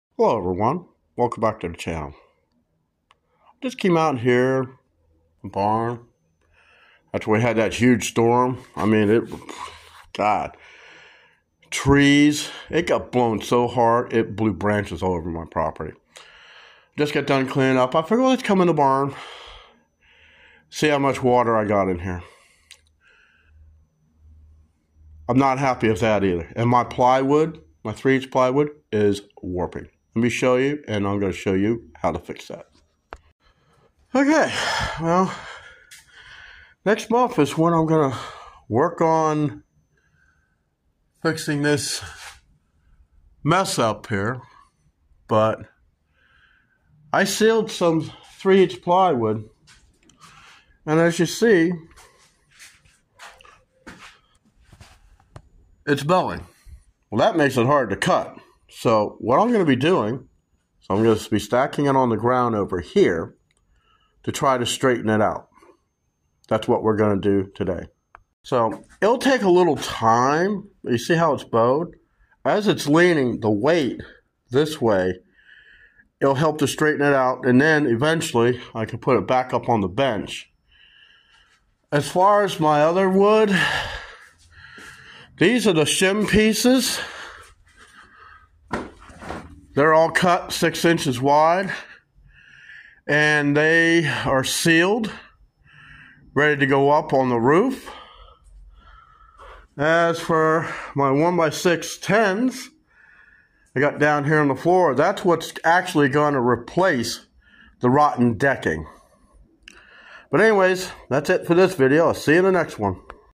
How to straighten warped plywood
Sometimes wood will warp in damp places and there's many ways to fix the problem.